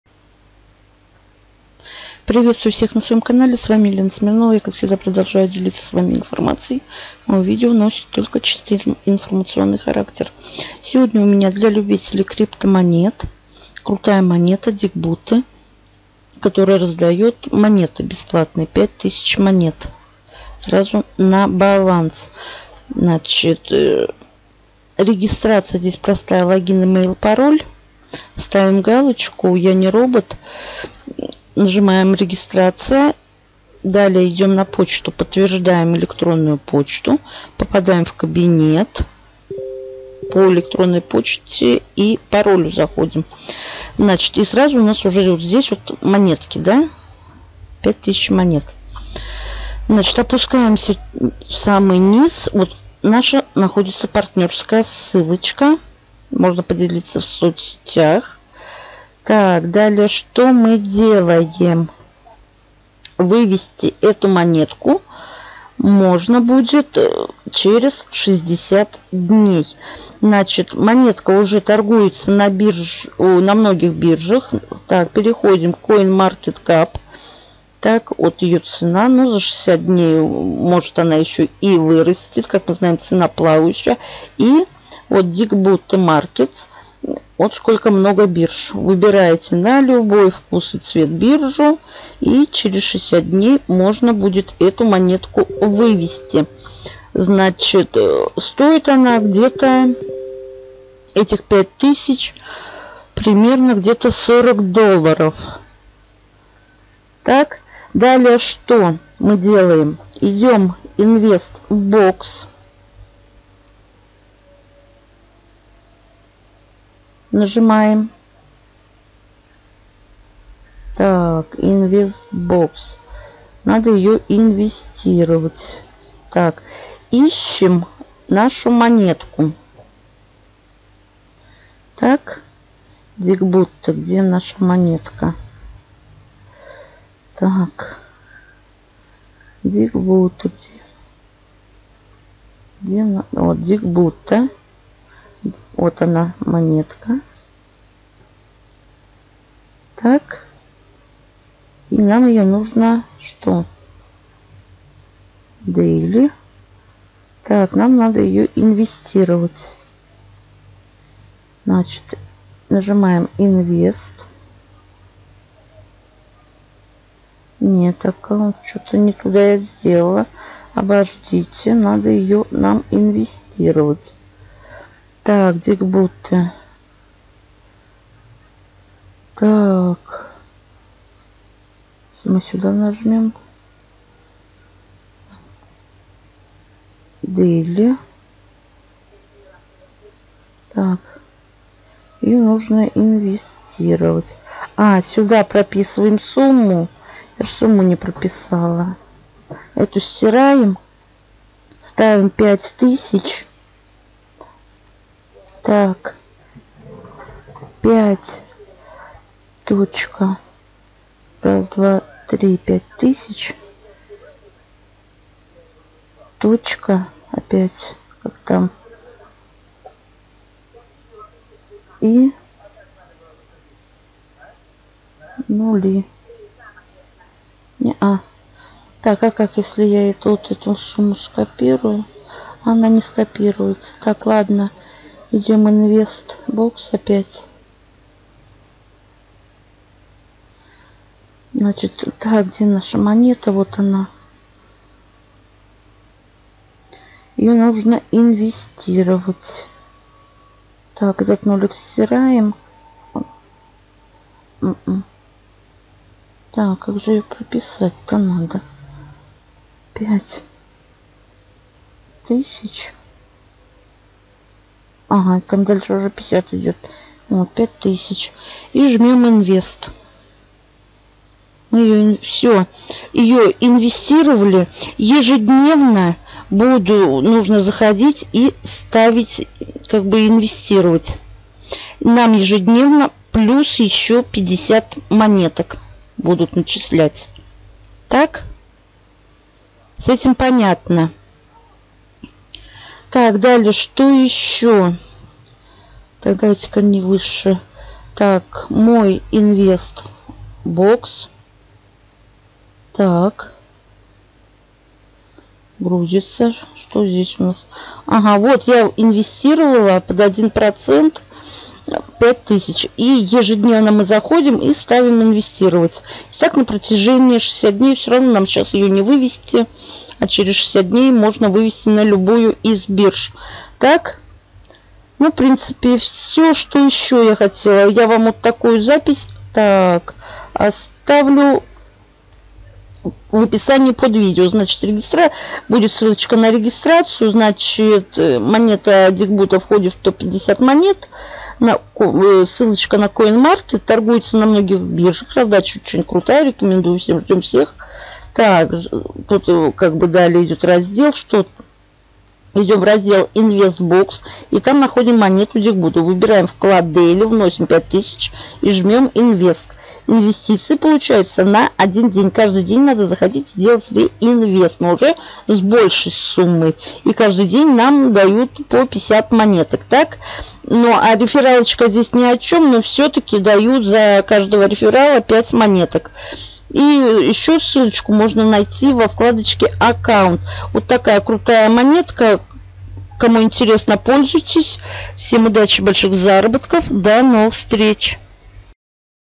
Крутая раздача 5000 DGB
Монета DGB входит в топ 50 всех монет Торгуется на многих биржах. Раздача очень крутая, рекомендую всем.
Жмем сверху Registration- регистрируемся и активируем аккаунт. После получаем 5000 DGB. Далее идем в раздел INVESTBOX и там находим монету DGB, выбираем вклад Daily, вносим 5000 и жмем Invest.
Инвестиции получаются на 1 день. Каждый день надо заходить и делать реинвест, но уже с большей суммой( каждый день падает по 50 DGB).